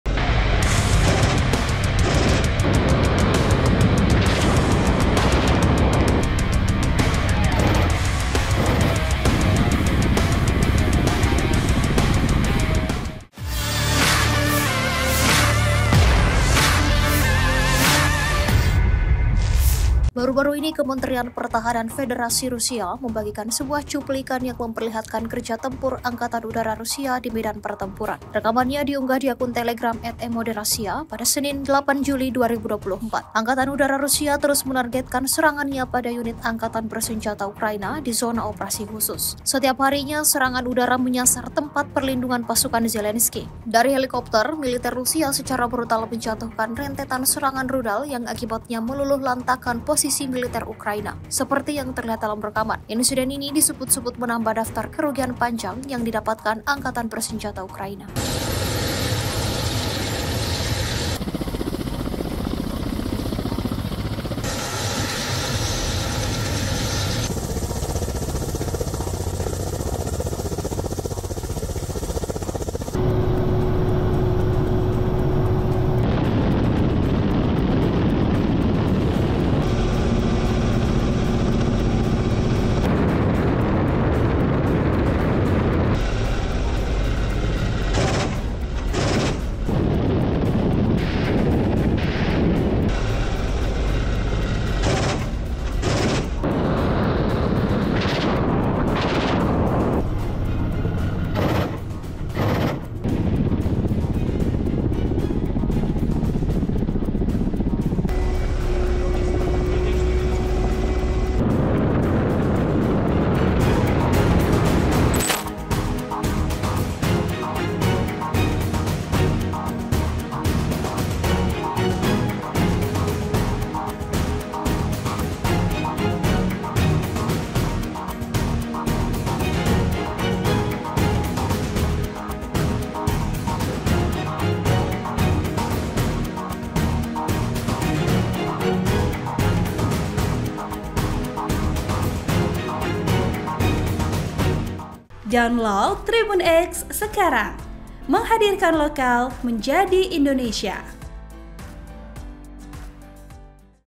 VIRAL!! Angkatan Udara Rusia Libas Pertahanan Musuhnya di Zona Operasi Khusus
TRIBUN-TIMUR.COM - Baru-baru ini Kementerian Pertahanan Federasi Rusia membagikan sebuah cuplikan yang memperlihatkan kerja tempur Angkatan Udara Rusia di medan pertempuran.

Rekamannya diunggah di akun Telegram @mod_russia pada Senin (8/7/2024).

Angkatan Udara Rusia terus menargetkan serangannya pada unit Angkatan Bersenjata Ukraina di zona operasi khusus.

Setiap harinya, serangan udara menyasar tempat perlindungan pasukan Zelensky.

Dari helikopter, militer Rusia secara brutal menjatuhkan rentetan serangan rudal yang akibatnya meluluh lantakkan posisi militer Ukraina.

Insiden ini disebut-sebut menambah daftar kerugian panjang yang didapatkan Angkatan Bersenjata Ukraina.

Editor Video: Muhamad Asrul
Naskah: Memet Fani Gunawan
Narator: Andi Dinda (Mahasiswa Magang UNM)

(TRIBUN-TIMUR.COM)